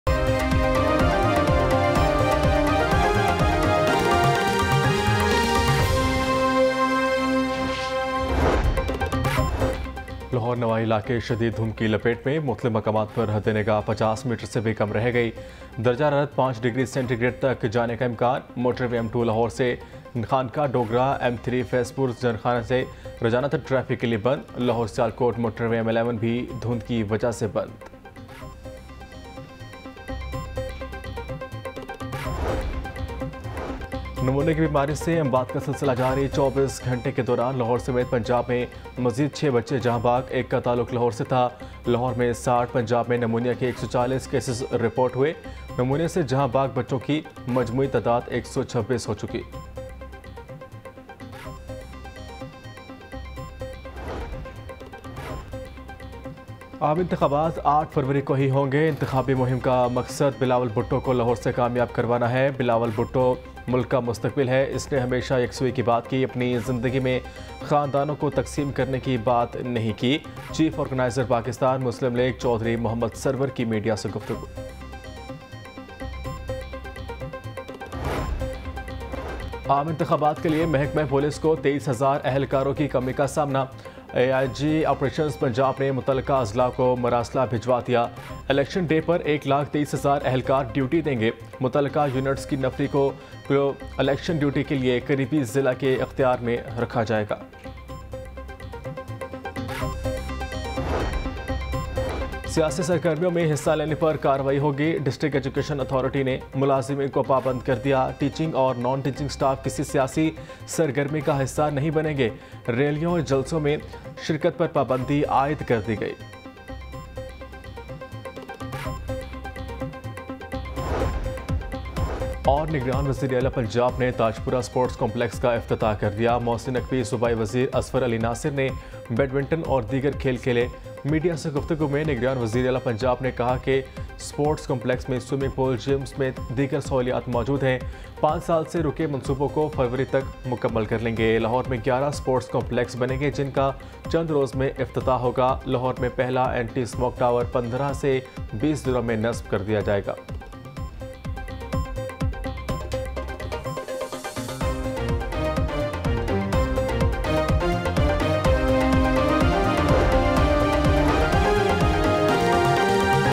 Siasi Sargarmiyon Mei Taizi | Lahore News Headlines 04 AM | 19 Jan 2024
Chicken Price Increase

Important visit of caretaker CM Mohsin Naqvi

Students Alert!!

Eggs Price Increases

Good News For PML-N!!

Attacks of Dengue Increased

Bad news!! Vegetables become more expensive

Weather of Lahore became cold

Lahore High Court's Big Order

Poor people are worried about inflation